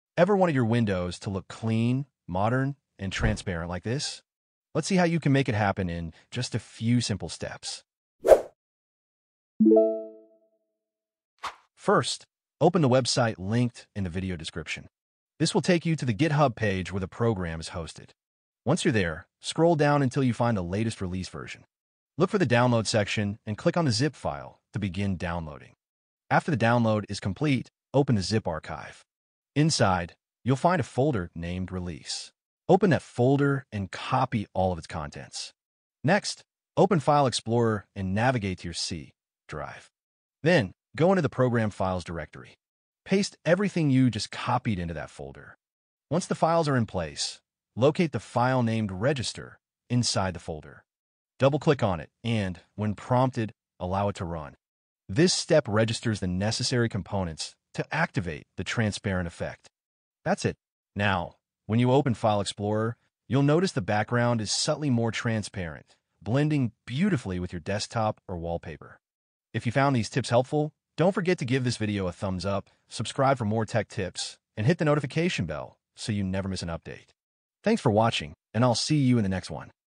Make Your Windows Look Transparent – Easy Trick
Make Your Windows Look Transparent – Easy Trick | Transparent File Explorer Windows 10/11

Want to give your Windows a sleek, modern look with a transparent File Explorer? In this video, I’ll show you a super simple method to make your Windows background look clean and stylish using a small, free tool from GitHub.

✨ What You’ll Learn:

How to make File Explorer transparent on Windows 10 & 11

Step-by-step setup tutorial (easy for beginners!)

How to download, install, and activate the effect

Make your desktop look minimalist and futuristic

📌 No coding required – just follow along and get that transparent glass effect in under 5 minutes.

If you enjoy making your desktop more aesthetic, don’t forget to: 👍 Like the video
💬 Comment if you have any questions or suggestions!